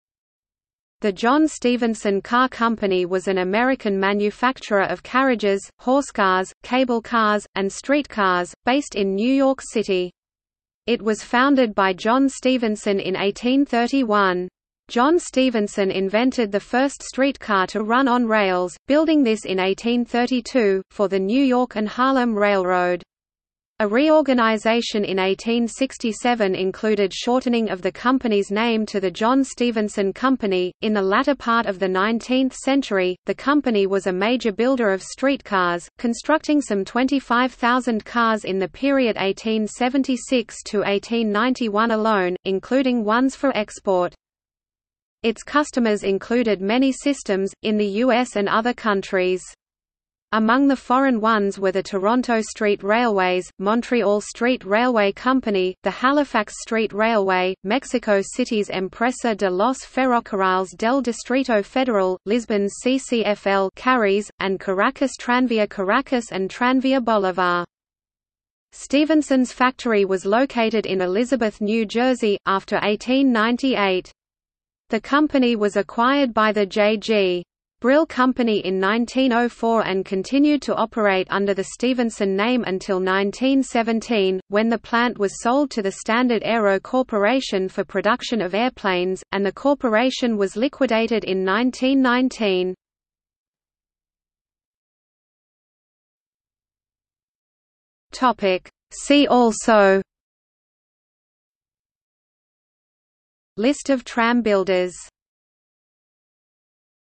John Stephenson Company | Wikipedia audio article
00:01:34 See also

- Socrates

SUMMARY
The John Stephenson Car Company was an American manufacturer of  carriages, horsecars, cable cars, and streetcars, based in New York City. It was founded by John Stephenson in 1831.  John Stephenson invented the first streetcar to run on rails, building this in 1832, for the New York and Harlem Railroad. A reorganization in 1867 included shortening of the company's name to the John Stephenson Company.  In the latter part of the 19th century, the company was a major builder of streetcars, constructing some 25,000 cars in the period 1876–1891 alone, including ones for export.
Its customers included many systems, in the US and other countries. Among the foreign ones were the Toronto Street Railways, Montreal Street Railway Company, the Halifax Street Railway, Mexico City's Empresa de los Ferrocarriles del Distrito Federal, Lisbon’s CCFL (Carris), and Caracas'  Tranvía Caracas and Tranvía Bolívar.

Stephenson's factory was located in Elizabeth, New Jersey, after 1898.  The company was acquired by the J.G. Brill Company in 1904 and continued to operate under the Stephenson name until 1917, when the plant was sold to the Standard Aero Corporation for production of airplanes, and the corporation was liquidated in 1919.